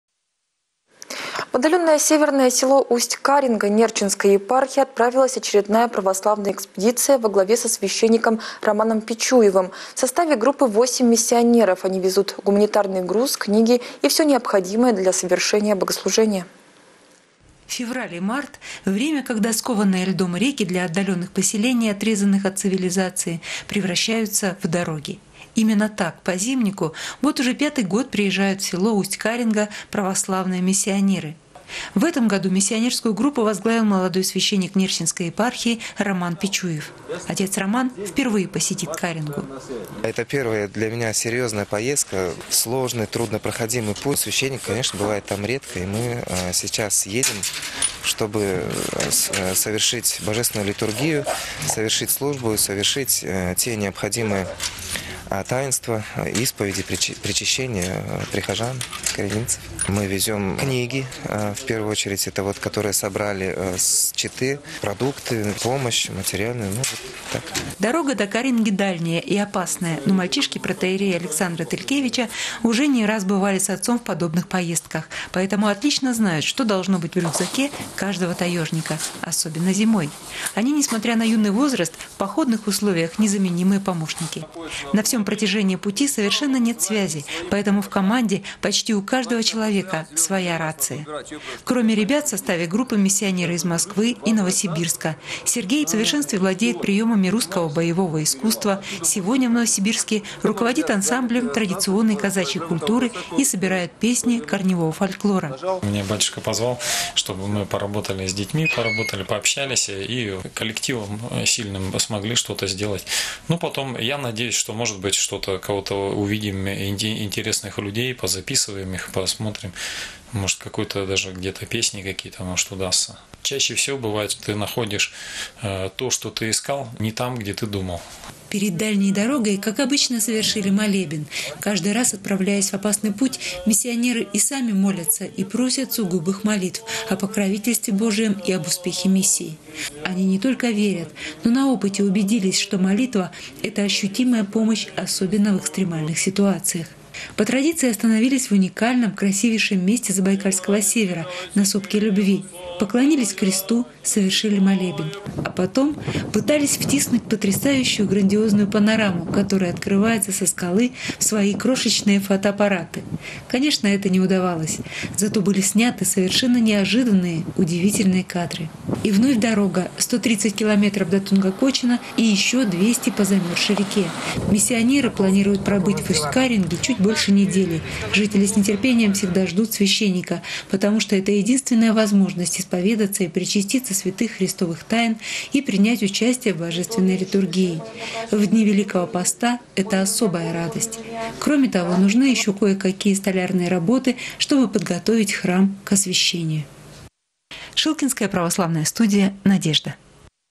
В северное село Усть-Каренга Нерчинской епархии отправилась православная экспедиция
В отдаленное северное село Усть-Каренга Нерчинской епархии отправилась очередная православная экспедиция во главе со священником Романом Пичуевым. В составе группы – 8 миссионеров. Они везут гуманитарный груз, книги и все необходимое для совершения богослужения.